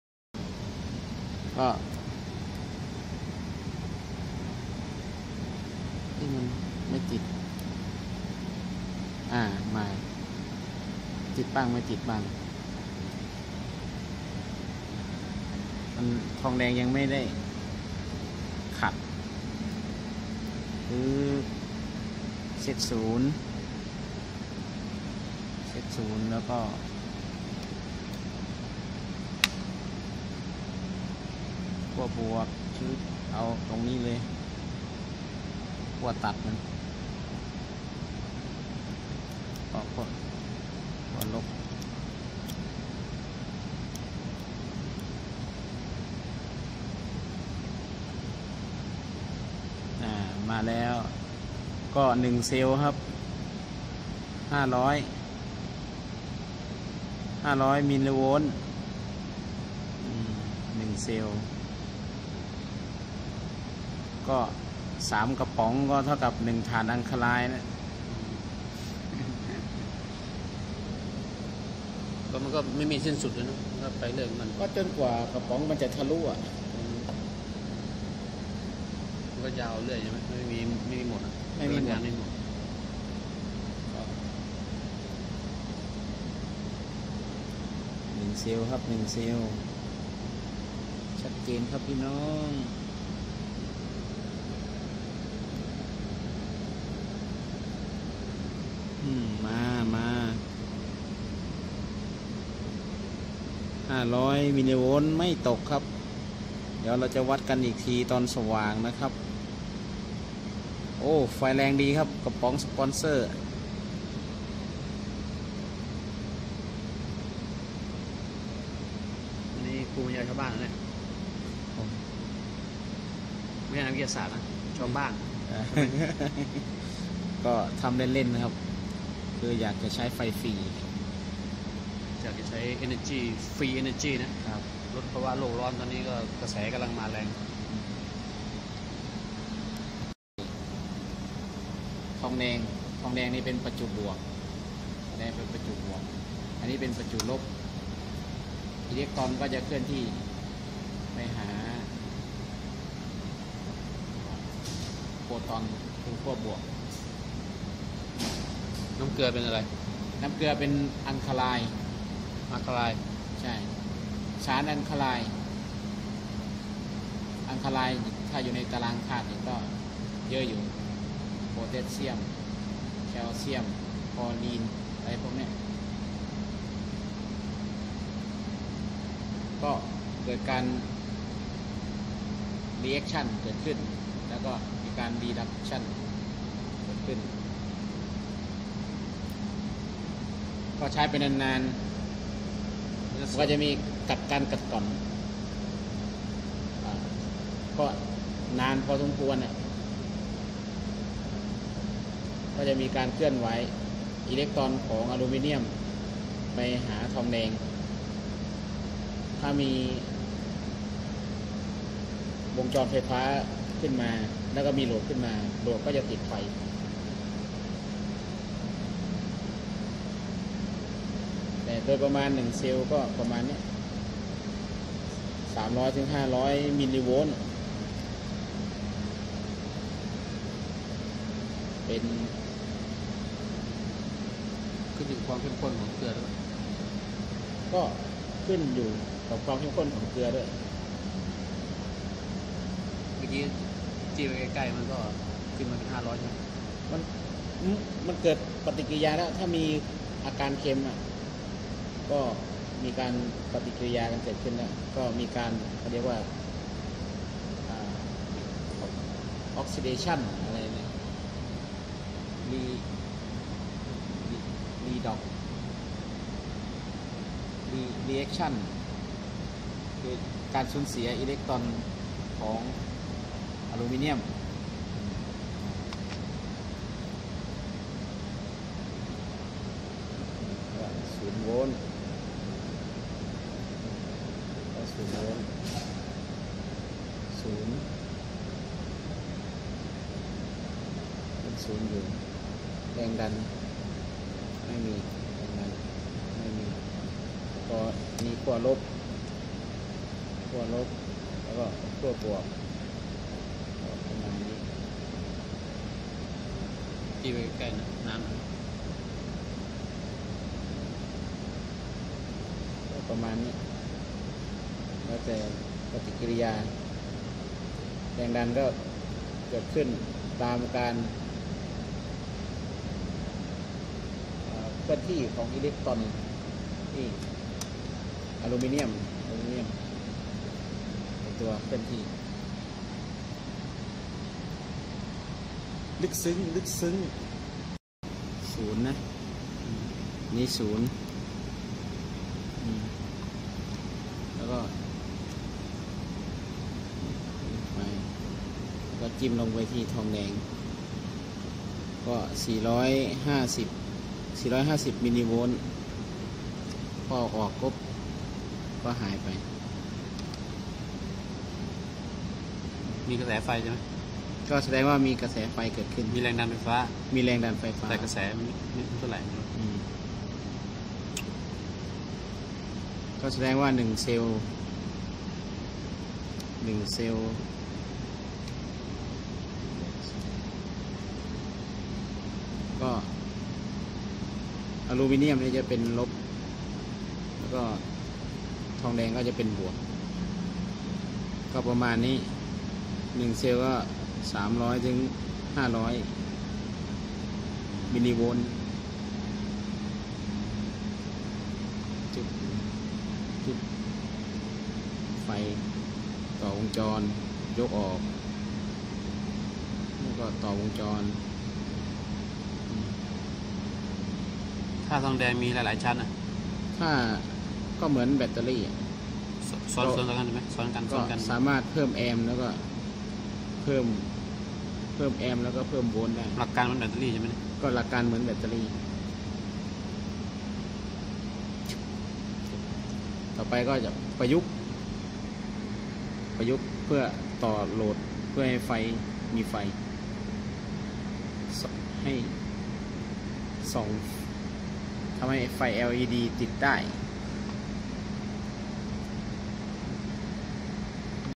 การทดลองผลิตไฟฟ้าจากน้ำเกลือ
น้ำเกลือสามารถใช้ผลิตไฟฟ้าได้